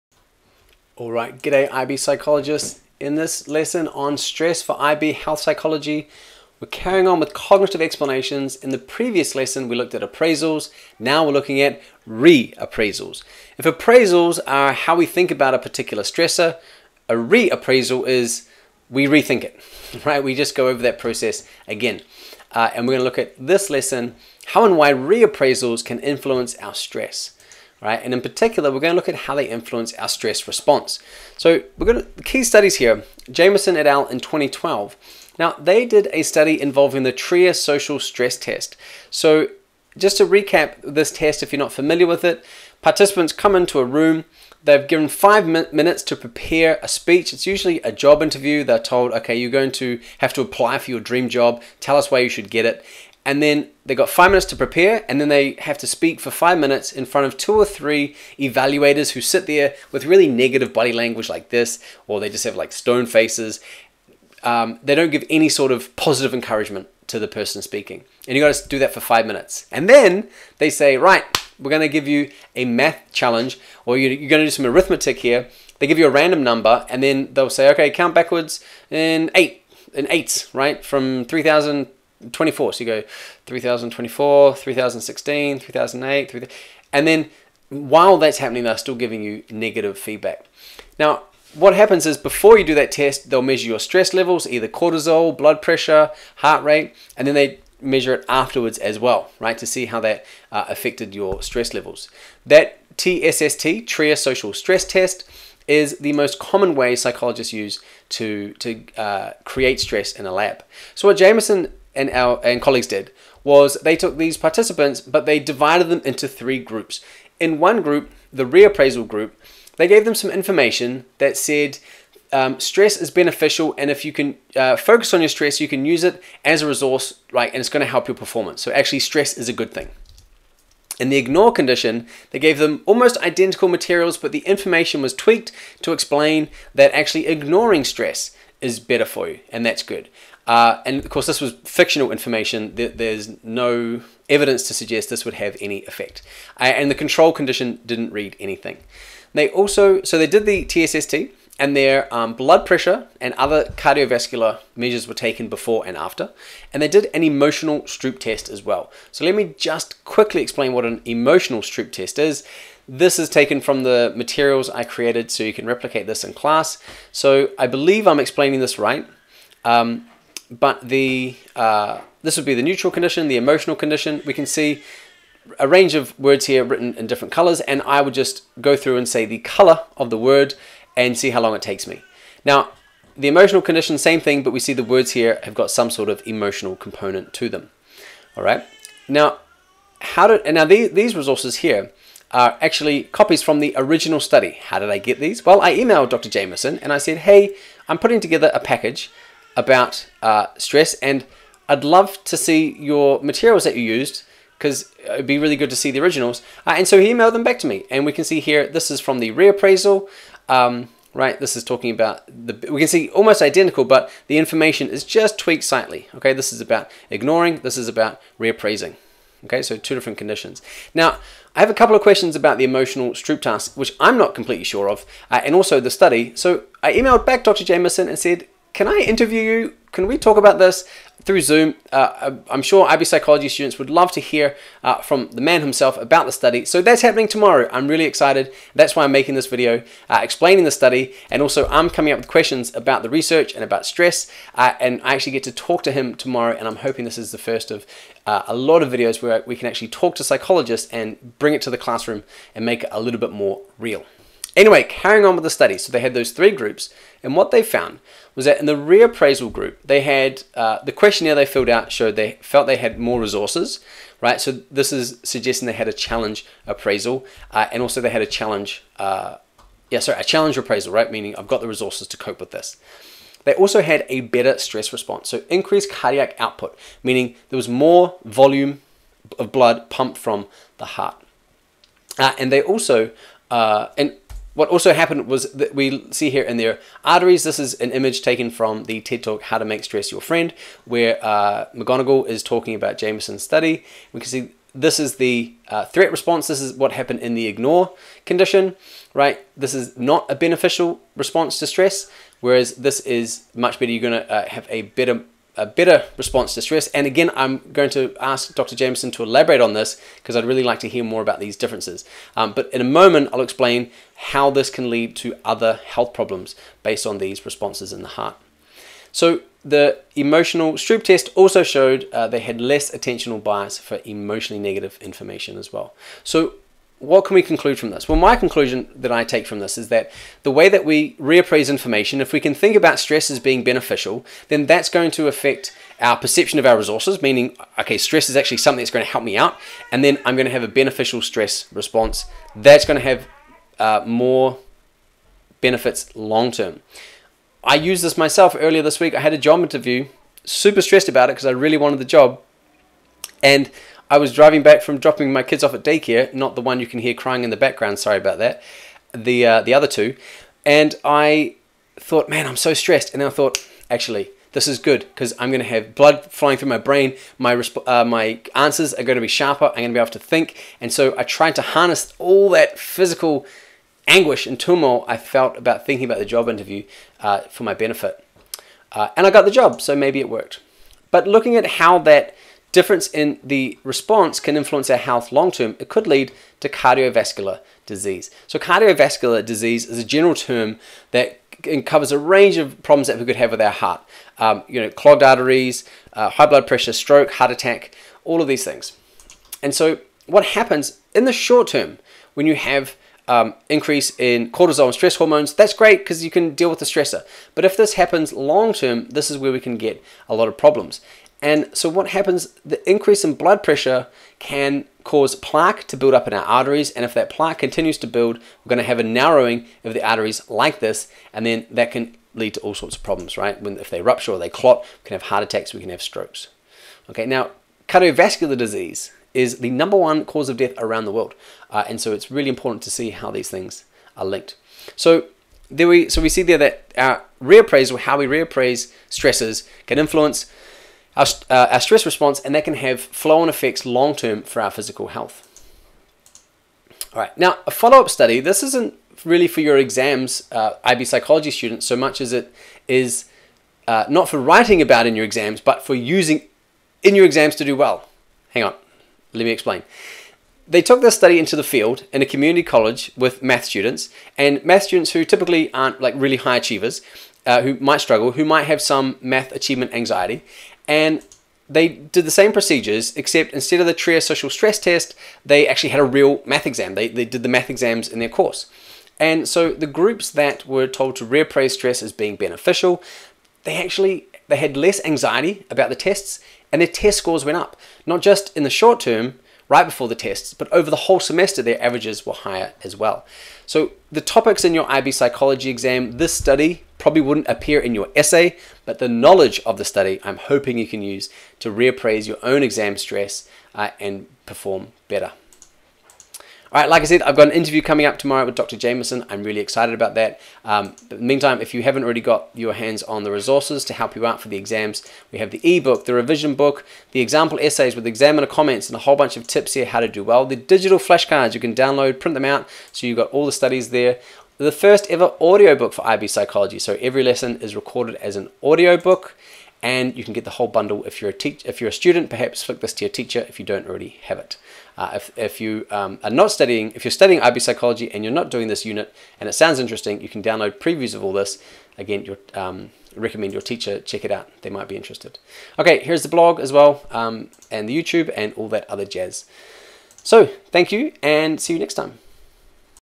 Cognitive reappraisals and Jamieson et al.'s key studies: IB Health Psychology Stress
This video tutorial accompanies Lesson 2b: RE-Appraisals in our unit for Stress: A Student's Guide to IB Health Psychology.

Cognitive reappraisals can influence our stress response, according to the biopsychosocial model of challenge and threat. This content can be really helpful for the BPS model in the IB Health Psychology option, as well as cognitive factors that affect stress.